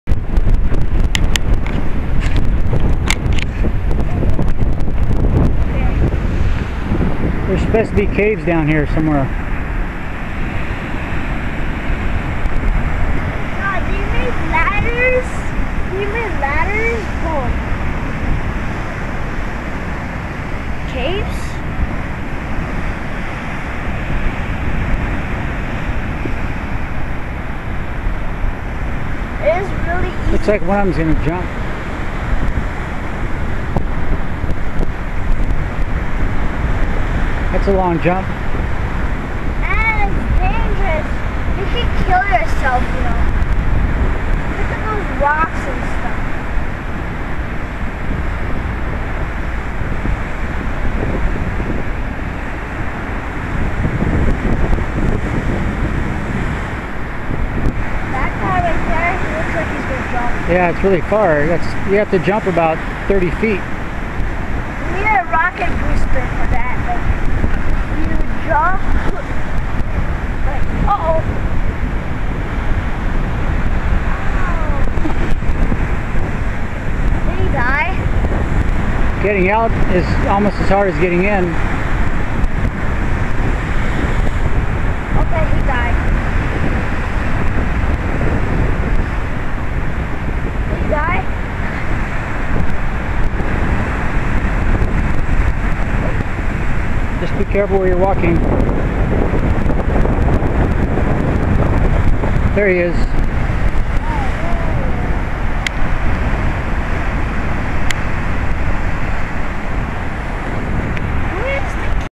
Koko Head and Spitting Caves Cliffs
Roma and I biked down Lumahai Street, then hiked down to Spitting Caves Cliffs on southeast Oahu, near KoKo Head